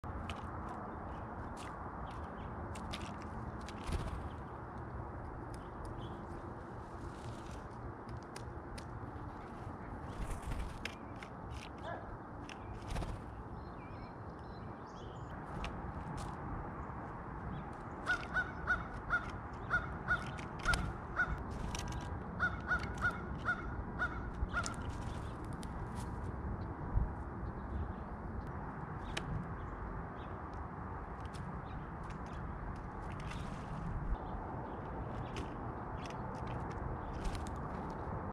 Blue Jay at Feeder
A couple clips from my GoPro I placed at my feeder for the Blue Jays